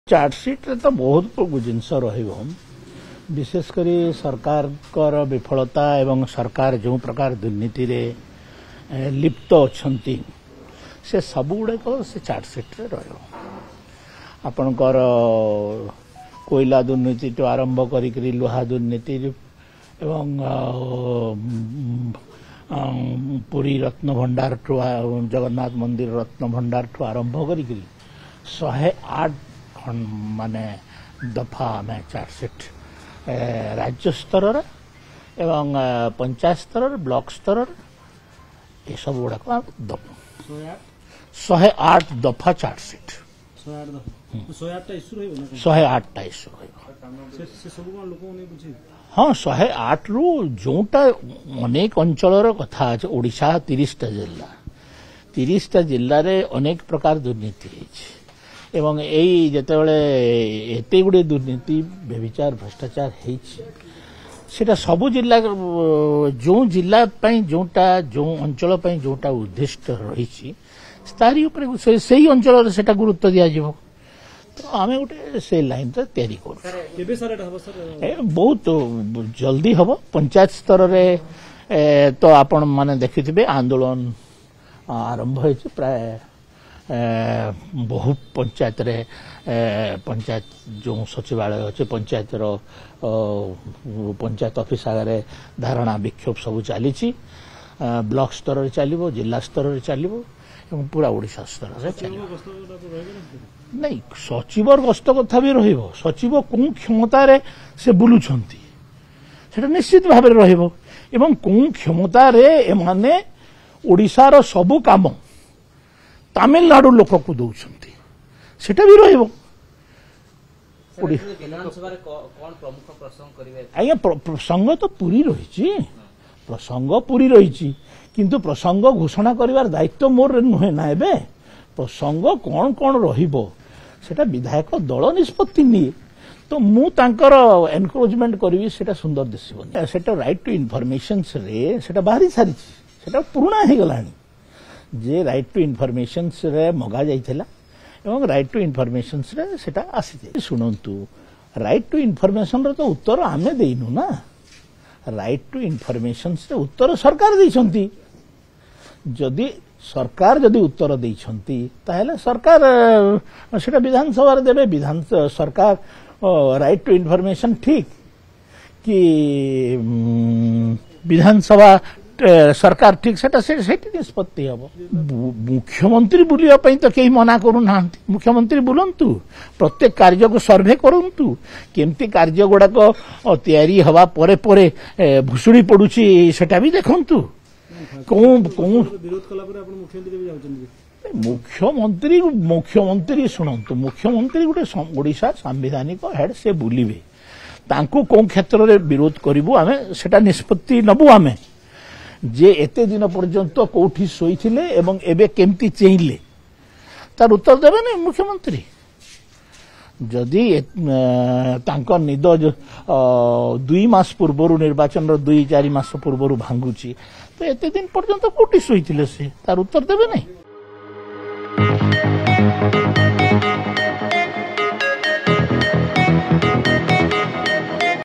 ‘ବିଜେପି ଆଣୁଛି 108 ଦଫା ଚାର୍ଜସିଟ୍’| Odisha Reporter
ରାଜ୍ୟର ବିଭିନ୍ନ 108ଟି ପ୍ରସଙ୍ଗକୁ ନେଇ ଦଳ ପ୍ରସ୍ତୁତ କରୁଛି ଚାର୍ଜସିଟ । 5-T ସଚିବଙ୍କ ଜିଲ୍ଲା ଗସ୍ତ ପ୍ରସଙ୍ଗ,ସଚିବ କେଉଁ କ୍ଷମତାରେ ଜିଲ୍ଲାକୁ ଜିଲ୍ଲା ବୁଲୁଛନ୍ତି । ଏହାସହ କେଉଁ କ୍ଷମତାରେ ଓଡ଼ିଶାର ସବୁ କାମ ତାମିଲନାଡୁ ଲୋକଙ୍କୁ ଦେଉଛନ୍ତି । ଏ ପ୍ରସଙ୍ଗ ମଧ୍ୟ ଚାର୍ଜସିଟରେ ରହିବ ।